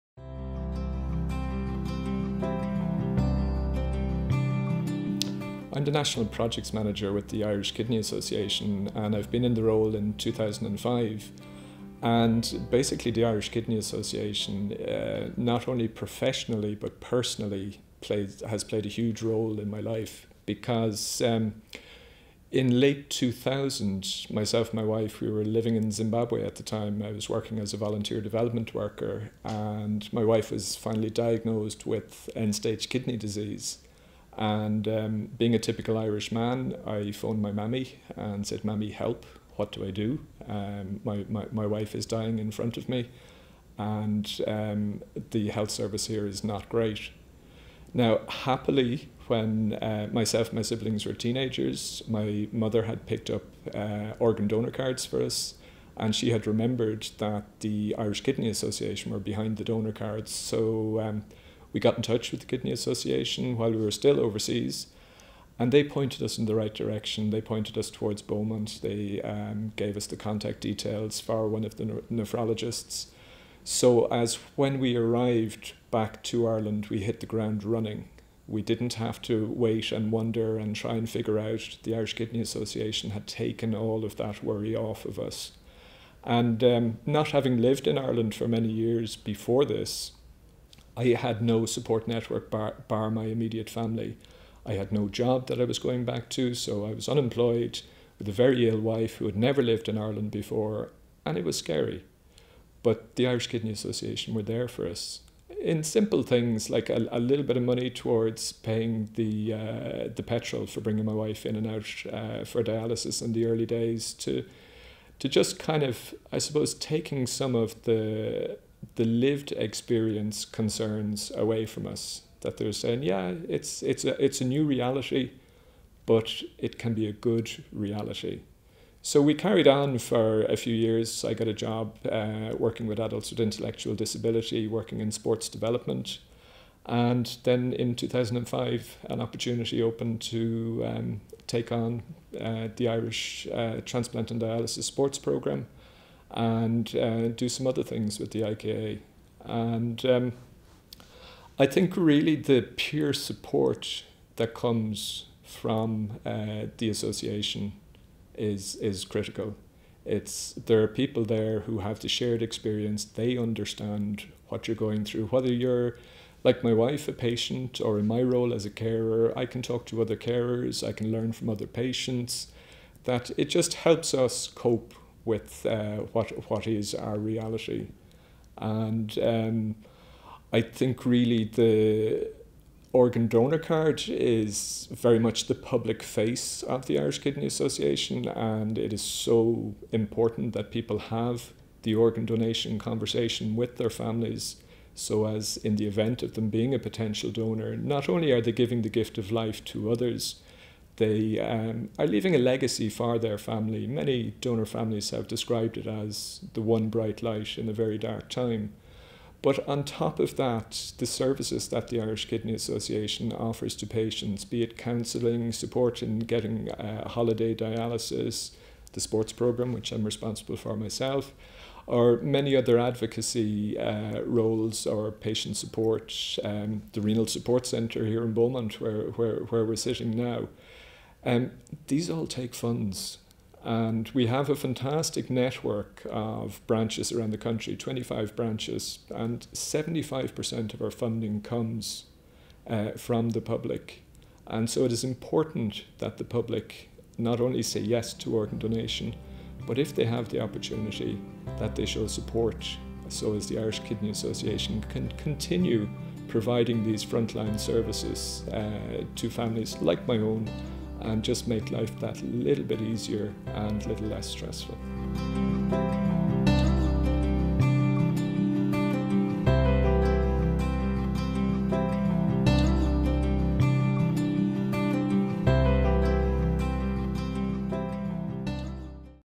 Colin White, Husband of Dialysis Patient Chikoyo
Colin, who is the IKA's National Projects Manager, talks about his wife Chikoyo and the support that the IKA offered them.